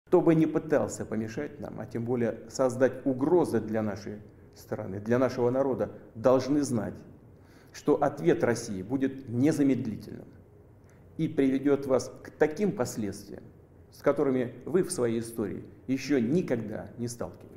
Ucraina, Putin: "Ritorsioni mai sperimentate se qualcuno interferisce"
Le parole del presidente russo dopo aver lanciato l'operazione militare nel Paese

 (Mosca). Il presidente Putin promette ritorsioni contro chi interferisce con l'operazione russa in Ucraina. "Chiunque cerchi di interferire , e ancora di più di minacciare il nostro Paese, il nostro popolo deve sapere che la risposta della Russia sarà immediata e vi porterà a tali conseguenze che non avete mai sperimentato nella vostra storia". (Redazione)